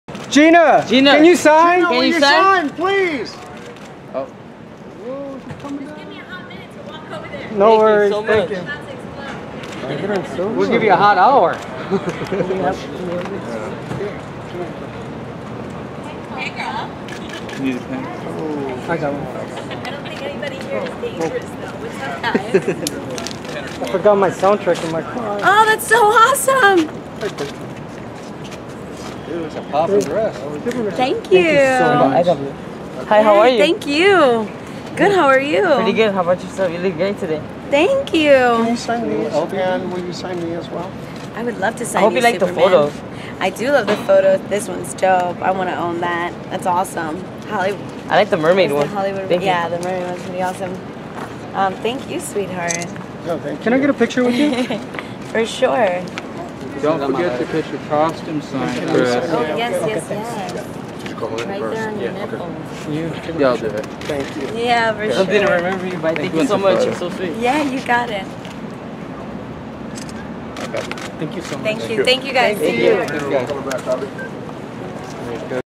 Jane The Virgin actress Gina Rodriguez shares the love with fans outside Jimmy Kimmel live
Jane the Virgin actress Gina Rodriguez loses her talk show virginity appearing on the Jimmy Kimmel live show for the first time.  Gina was super nice to her waiting fans!  She even signed for Hollywood character Superman's nipple